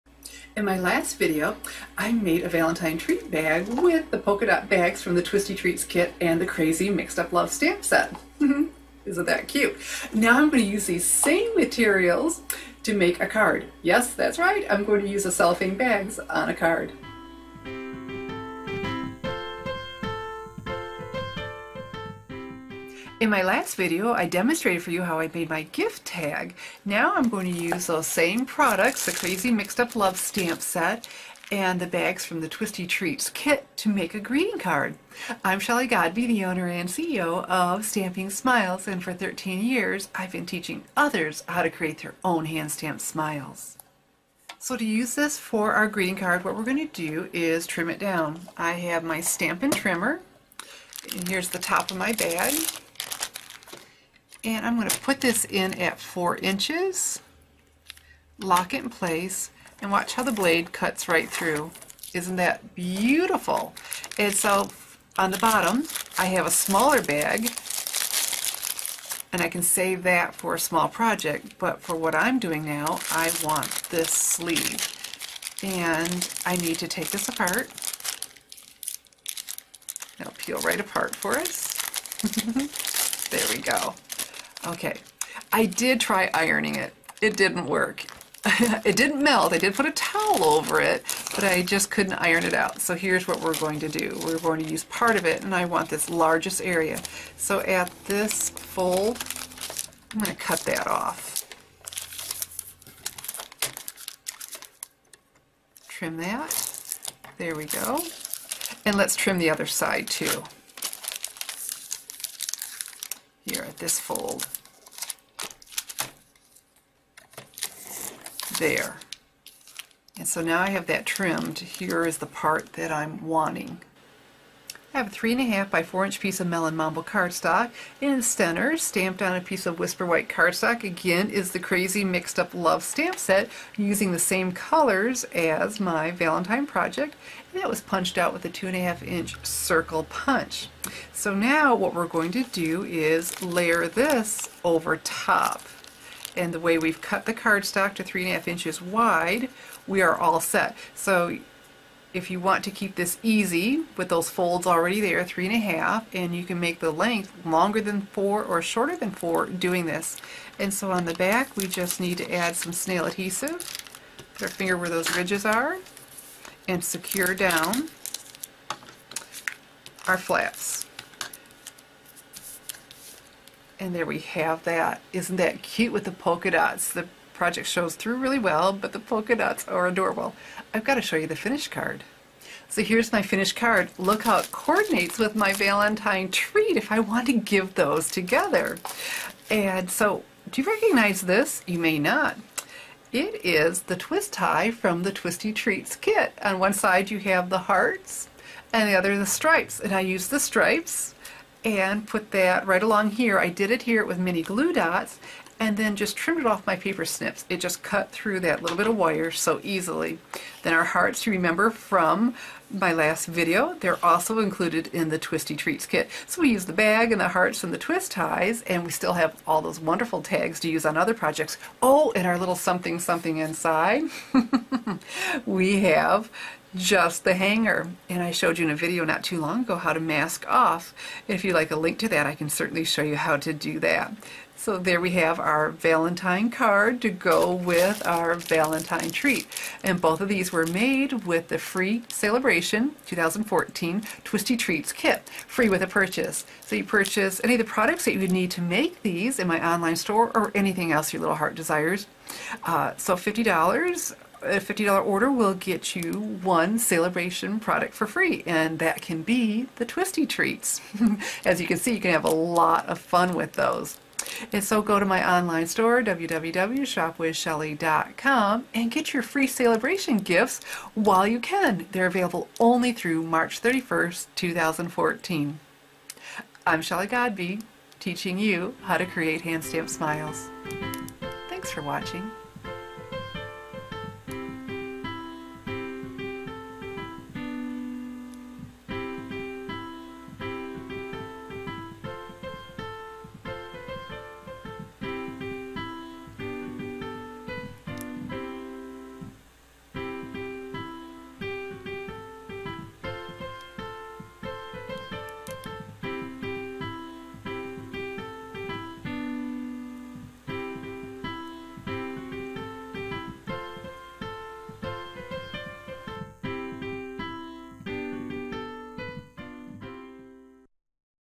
Make a Stampin' Up! Valentine's Day Card with Crazy, Mixed-Up Love
The Stampin' Up! Crazy, Mixed-Up Love Stamp Set can be used to make quick and cute Valentine's Day cards.  Watch my video to see how I also used the polka dot bags from the Stampin' Up! Sale-a-bration Twisty Treats Kit for my Valentine's Day card.

to get your FREE Sale-a-bration gift with a $50 purchase (before shipping and sales tax) when placed by March 31, 2014!